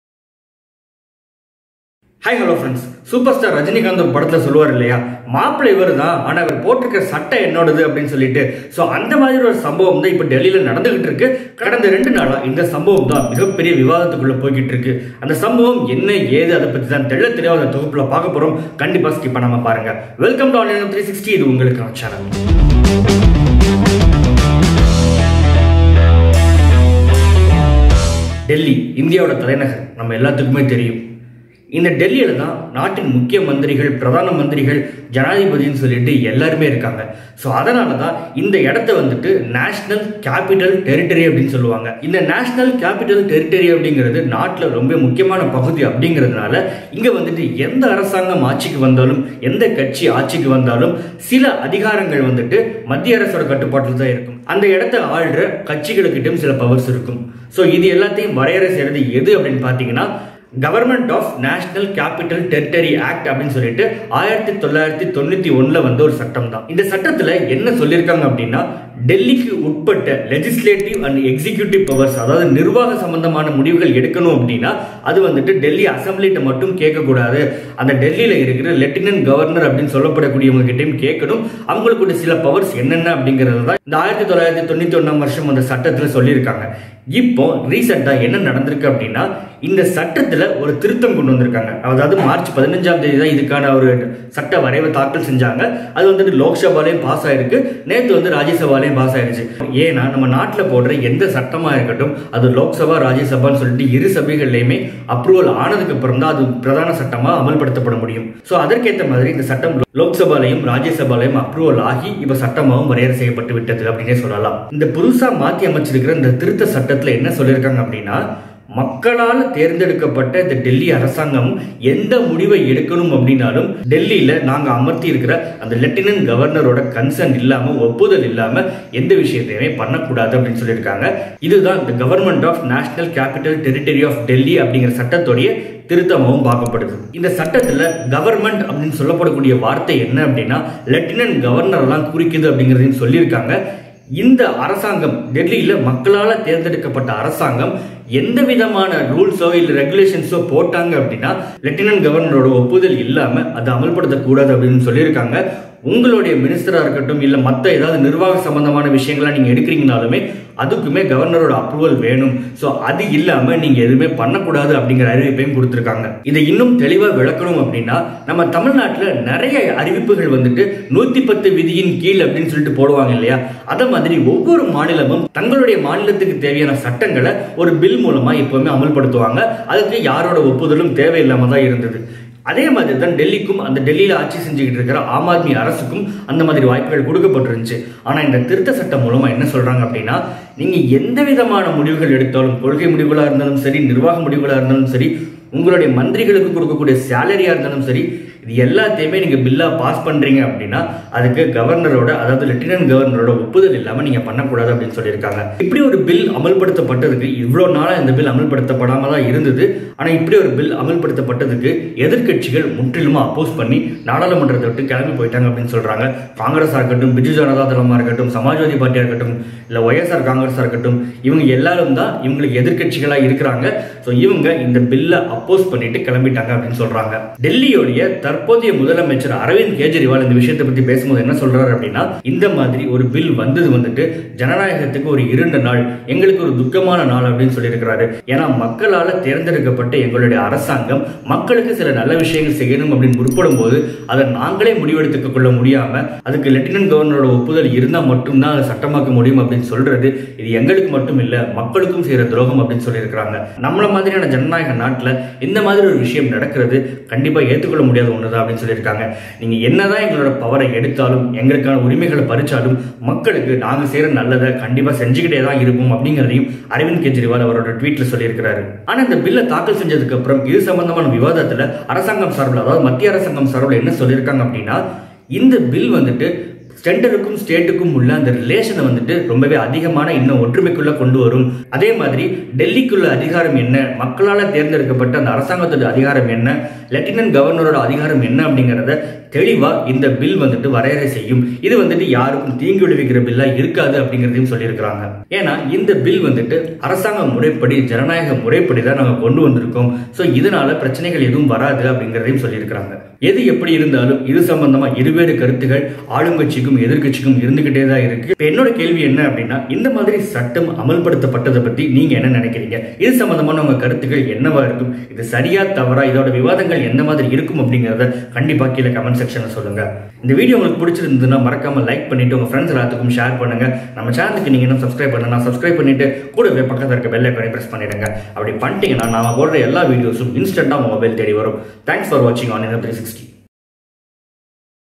Delhi Issue Explained | What's Happening ? | GNCTD bill 2021 | Arvind Kejriwal | Tamil
We are explaining about the Ongoing issue in Delhi

CLICK TO KNOW MORE!